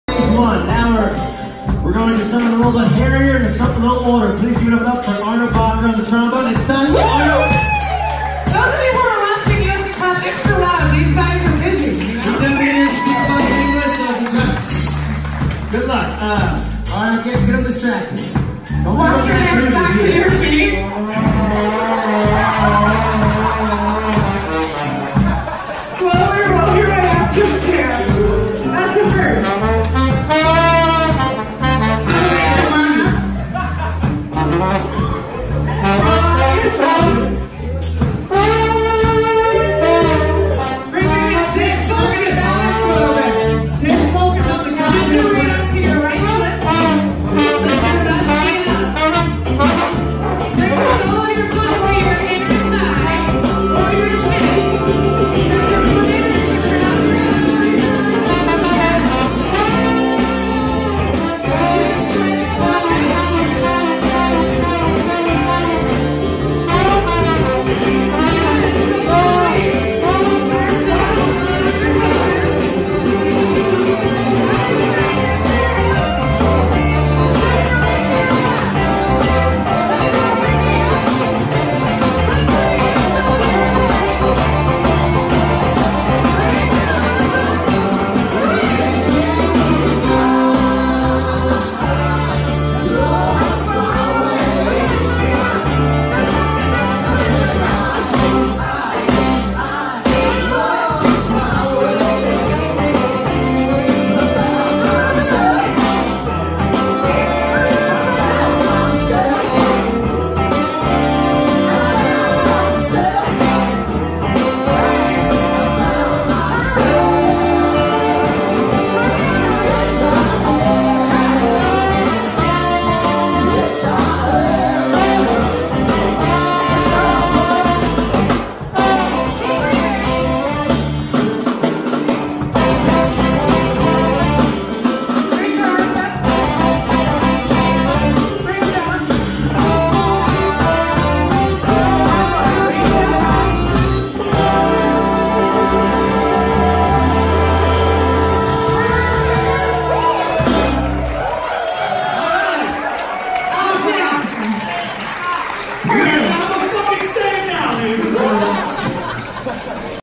Andy Frasco and the U.N.- Jam Cruise 17 - relaxing Yoga with a bone attack - January 18, 2019
Attack of the bone.. yo bro keep your bone to yourself